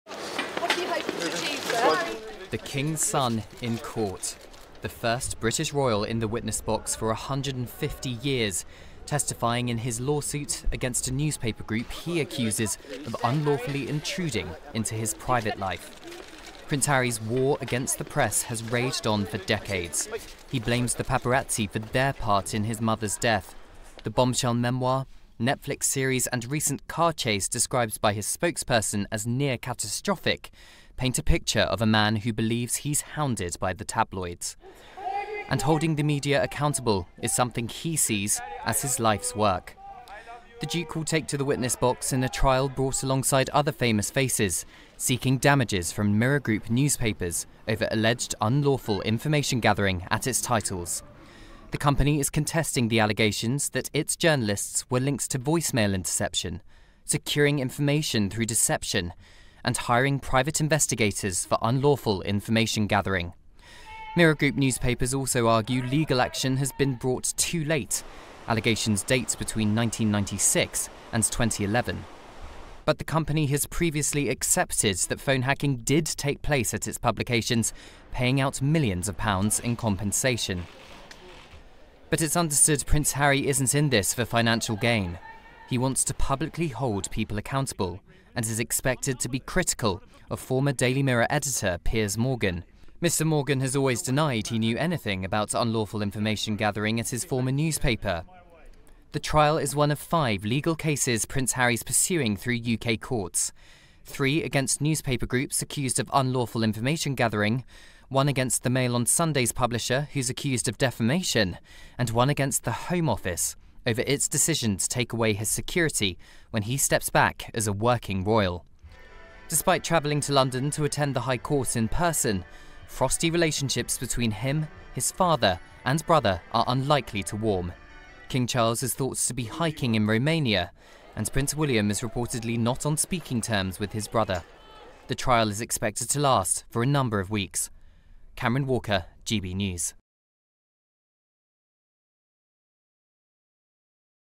Prince Harry to make history with UK court testimony | Cameron Walker reports
Prince Harry will return to London this week becoming the first British royal since the 19th century to give evidence in a London courtroom.

The Duke of Sussex is seeking damages from Mirror Group Newspapers over alleged unlawful information gathering at its titles.

Cameron Walker reports.